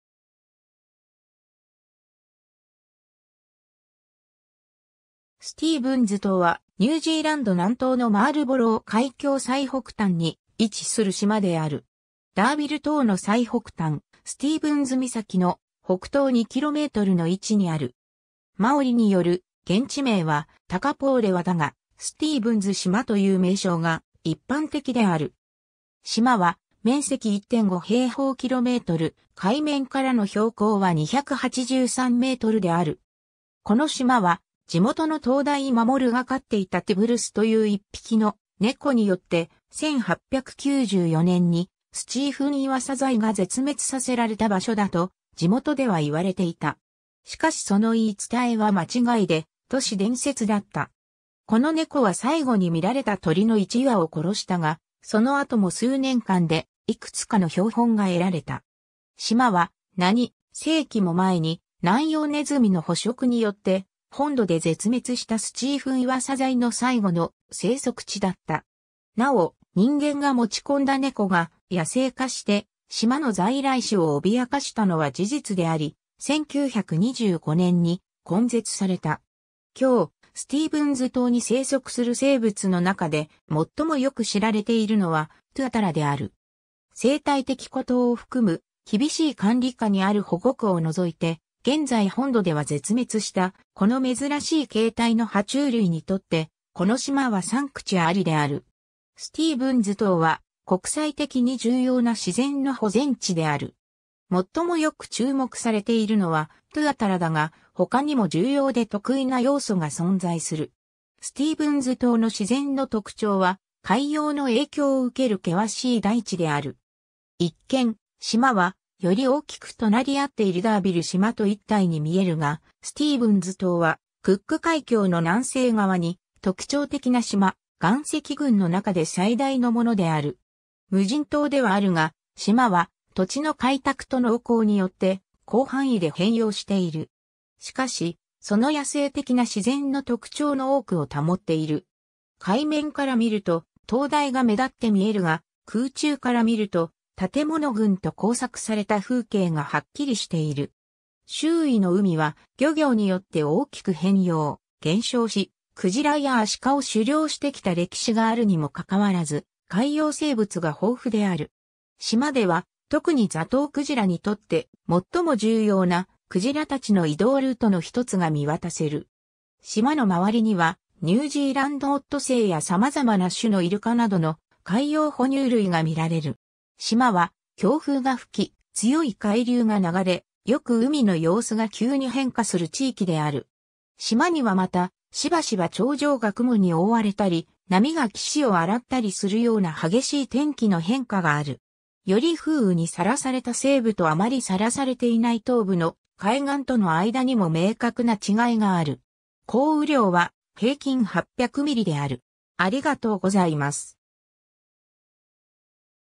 スティーブンズ島 (ニュージーランド)
スティーブンズ島（スティーブンズとう、英：Stephens Island）は、ニュージーランド南島のマールボロウ海峡最北端に位置する島である。
ダーヴィル島の最北端スティーブンズ岬の、北東2キロメートルの位置にある。
マオリによる現地名は「Takapourewa(カタカナ表記:タカポウレワ)」だが、スティーブンズ島という名称が一般的である。
島は面積1.5平方キロメートル（0.58平方マイル）、海面からの標高は283メートル (928 ft)である。
 この島は、地元の灯台守が飼っていたティブルス（英：Tibbles）という1匹の猫によって、1894年にスチーフンイワサザイが絶滅させられた場所だと、地元では言われていた。
しかしその言い伝えは間違いで 、都市伝説だった。
この猫は最後に見られた鳥の1羽を殺したが、その後も数年間で幾つかの標本が得られた。
島は、何世紀も前にナンヨウネズミの捕食によって本土で絶滅したスチーフンイワサザイの最後の生息地だった。
 なお、人間が持ち込んだ猫が野生化して島の在来種を脅かしたのは事実であり、1925年に根絶された。
 今日、スティーブンズ島に生息する生物の中で最もよく知られているのは、トゥアタラである。
生態的孤島を含む厳しい管理下にある保護区を除いて、現在本土では絶滅したこの珍しい形態の爬虫類にとって、この島はサンクチュアリである。
 スティーブンズ島は国際的に重要な自然の保全地である。
最もよく注目されているのはトゥアタラだが、他にも重要で特異な要素が存在する。
 スティーブンズ島の自然の特徴は海洋の影響を受ける険しい大地である。
一見、島は、より大きく隣り合っているダーヴィル島と一体に見えるが、スティーブンズ島はクック海峡の南西側に特徴的な島、岩石群の中で最大のものである。
無人島ではあるが、島は土地の開拓と農耕によって広範囲で変容している。
しかし、その野生的な自然の特徴の多くを保っている。
海面から見ると、灯台が目立って見えるが、空中から見ると、建物群と耕作された風景がはっきりしている。
 周囲の海は、漁業によって大きく変容、減少し、クジラやアシカを狩猟してきた歴史があるにもかかわらず、海洋生物が豊富である。
島では、特にザトウクジラにとって最も重要な、クジラたちの移動ルートの1つが見渡せる。
島の周りには、ニュージーランドオットセイや様々な種のイルカなどの海洋哺乳類が見られる。
 島は強風（平均風速14ノット、時速26キロまたは時速16マイル）が吹き、強い海流が流れ、よく海の様子が急に変化する地域である。
島にはまた、しばしば頂上が雲に覆われたり、波が岸を洗ったりするような激しい天気の変化がある。
より風雨にさらされた西部とあまりさらされていない東部の海岸との間にも明確な違いがある。
降雨量は平均800ミリ（31.5インチ）である。